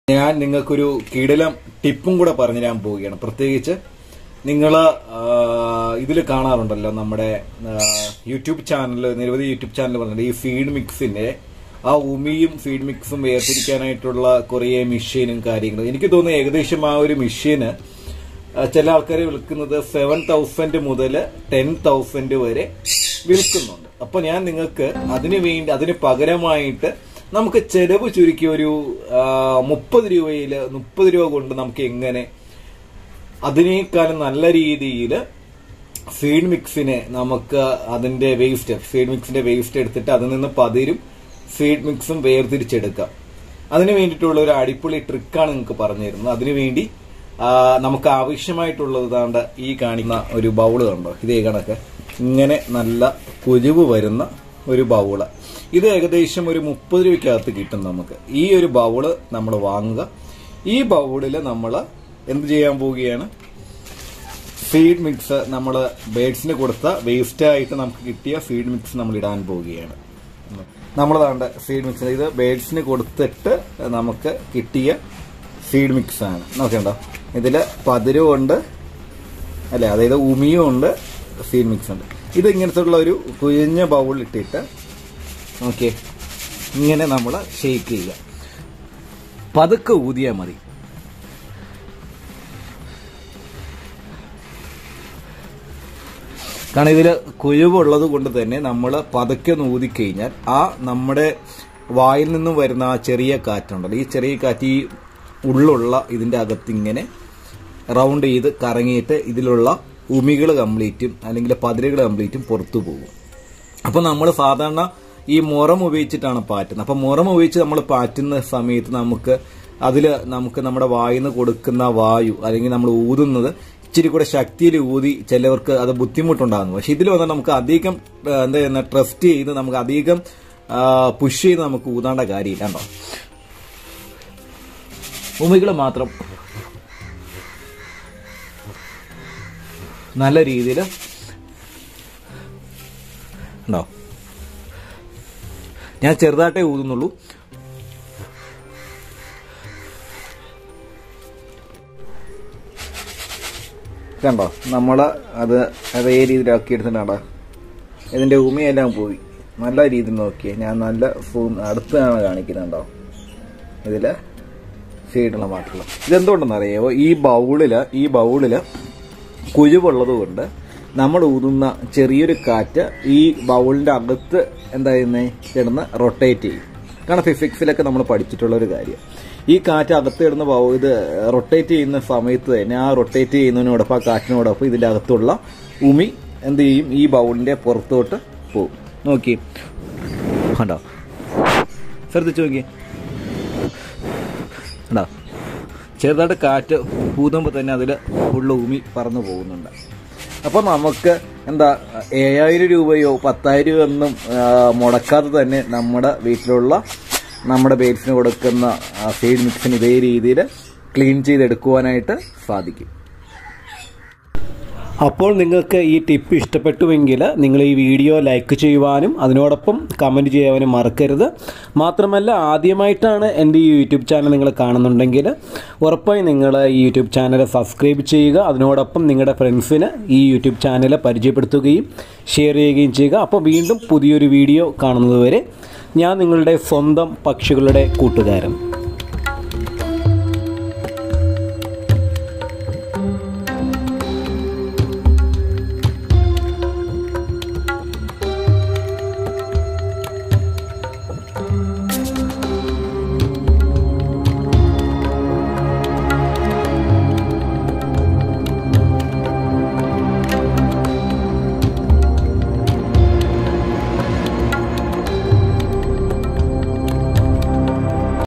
Birds Seed Cleaning Machine alternative Solution | low Budget Seed Cleaning | കാശ് ലാഭിക്കാൻ ഇത് മതി
Jr vlogs is all about aviary tours, birds and bird accessories
If you are a bird lover.. You are in the right place.